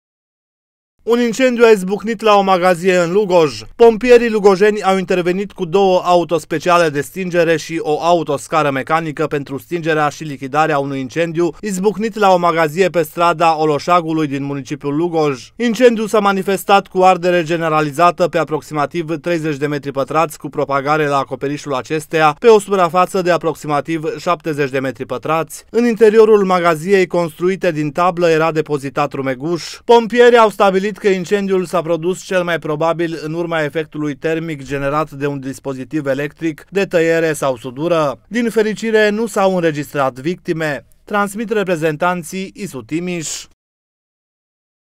INCENDIU ÎN BANAT
Un incendiu a izbucnit la o magazie în Lugoj. ”Pompierii lugojeni au intervenit cu două autospeciale de stingere și o autoscară mecanică pentru stingerea și lichidarea unui incendiu izbucnit la o magazie pe strada Oloșagului din municipiul Lugoj.